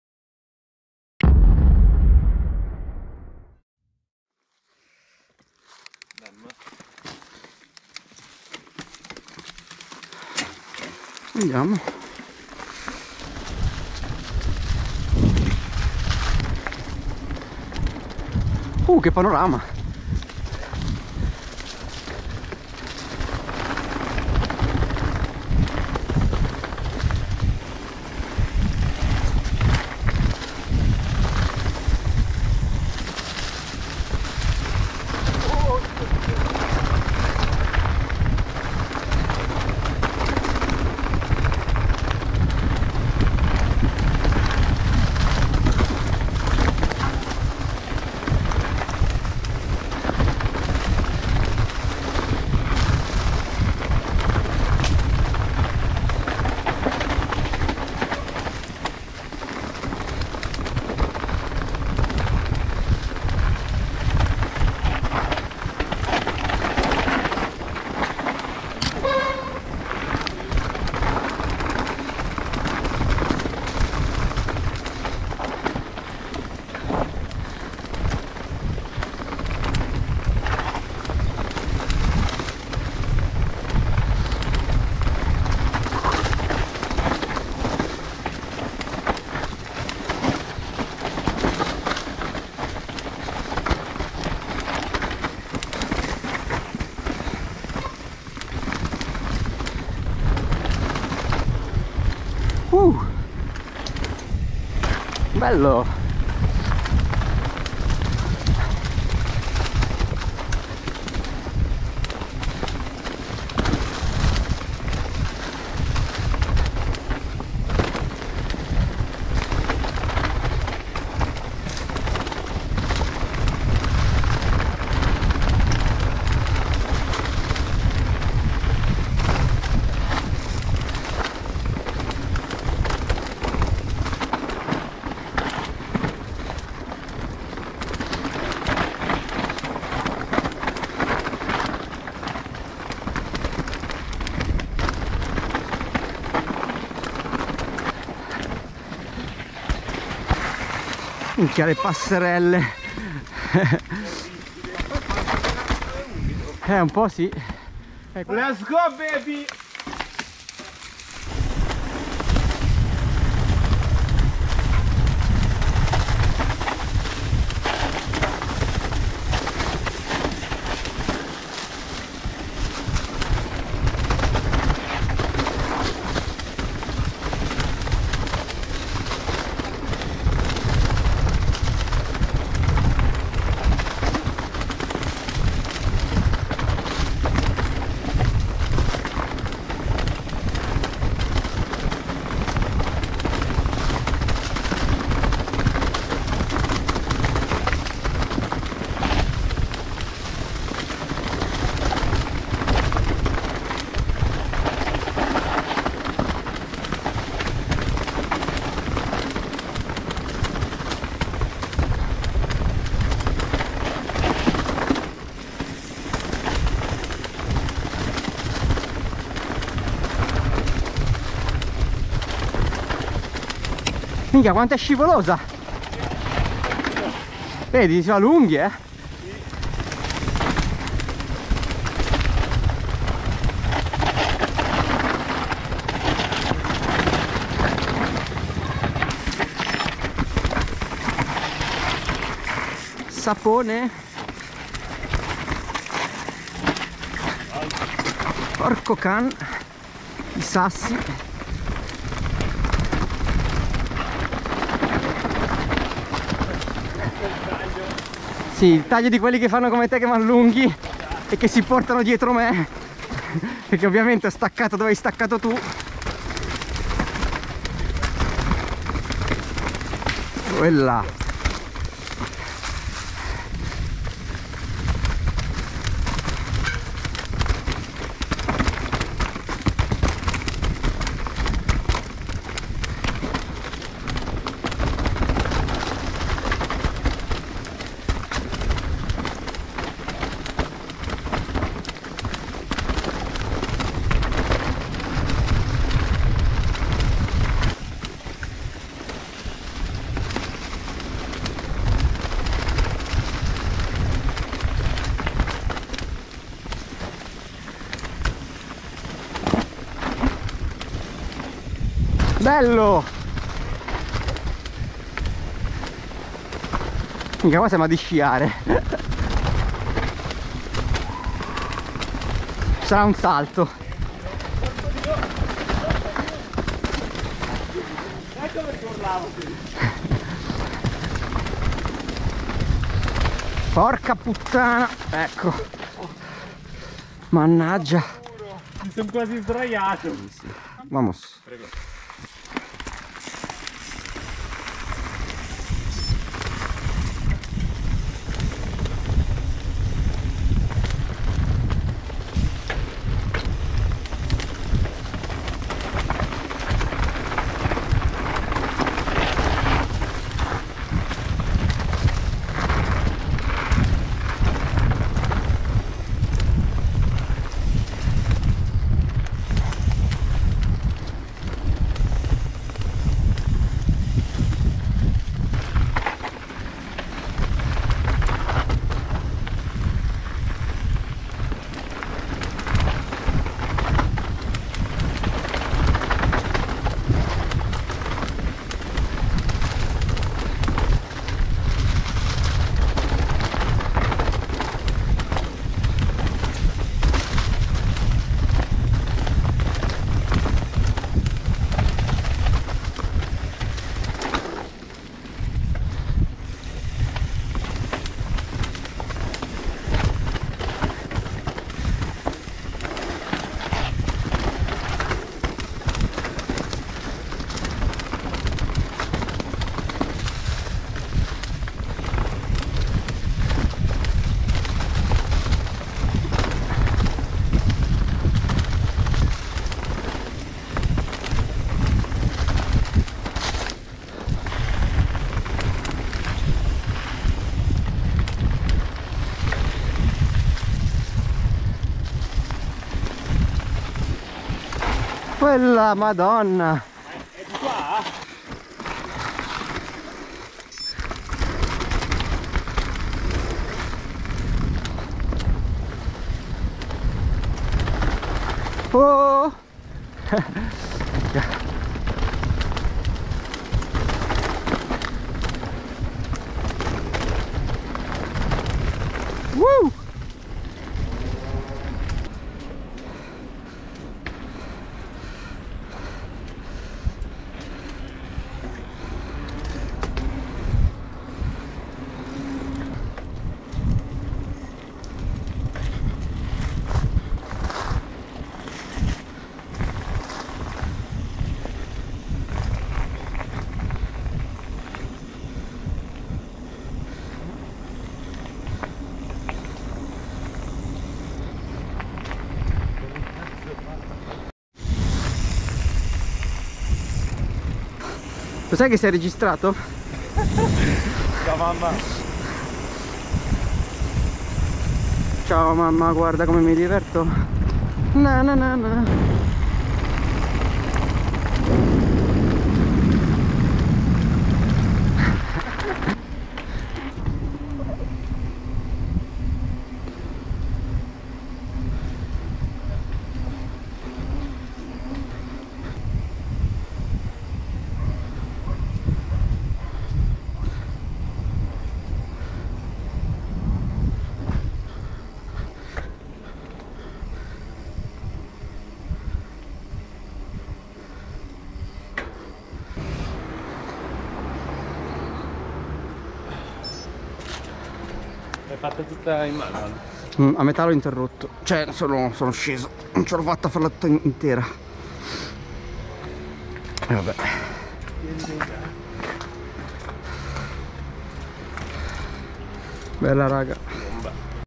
I Sentieri Dei Gatti - Gattinara - Mountain Bike
Prima volta a Gattinara guidando a vista! Trails molto divertenti!